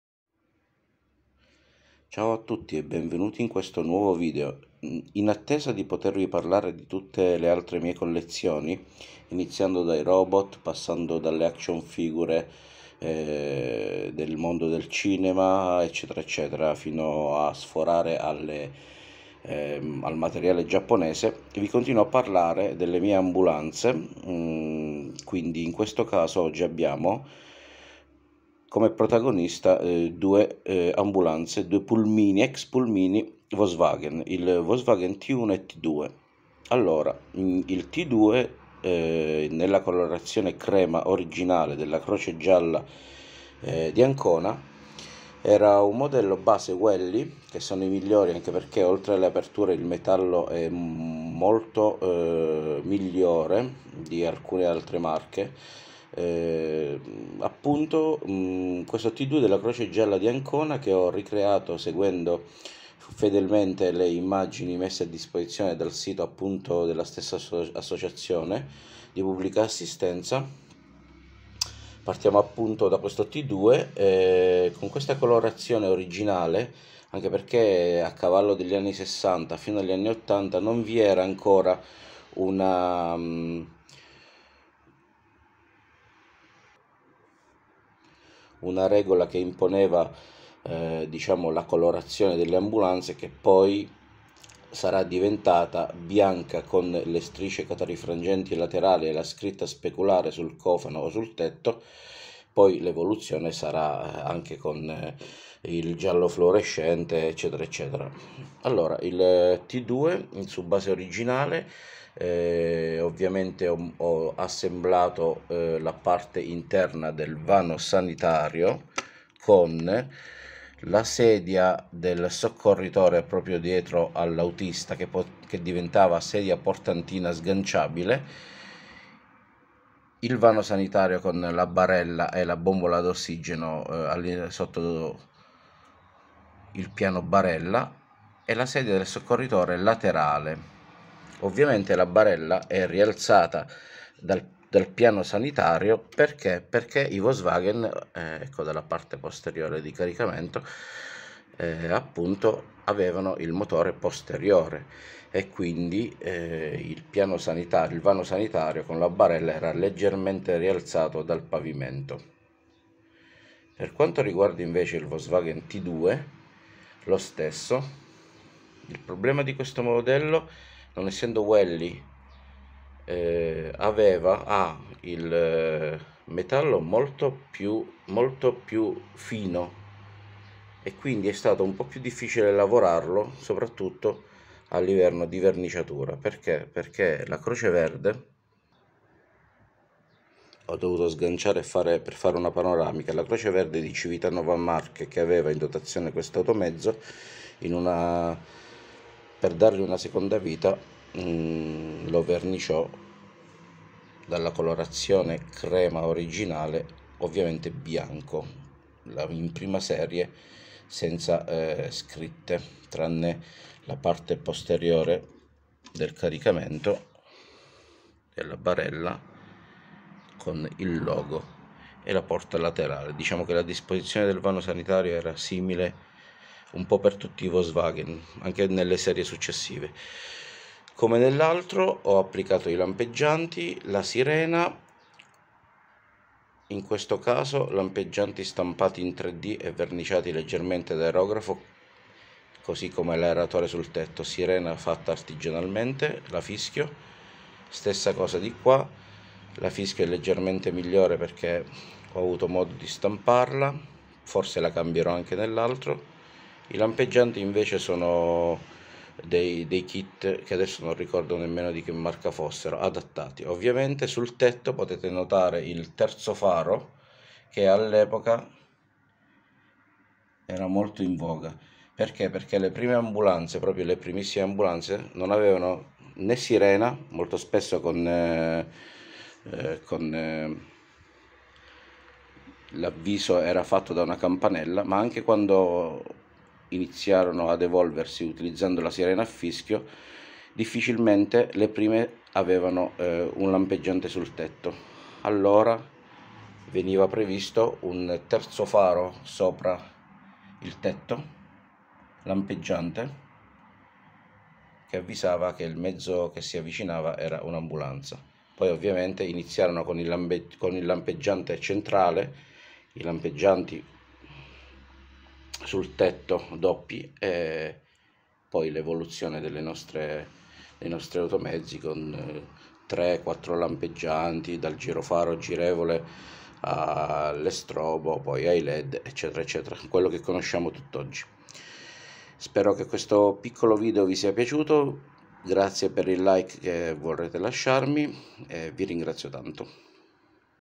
Volkswagen T1 e T2 Ambulanza
Ciao a tutti voi
Prima di iniziare a parlarvi di tutte le mie altre creazioni modellistiche e delle altre mie collezioni continuiamo con la serie "Ambulanze " in sc 1.24 - parlandovi questa volta di due automezzi che negli anni 70 e 80 andavano per la maggiore, il VW T1 e T2 che ho trasformato in ambulanza partendo da una parte su base Welly-Nex per il T2  e dall'altra Majsto per il T1...dove ho dovuto lavorare un po di piu ma...credo che tutto sommato sia venuto fuori un discreto risultato!!
Maurizio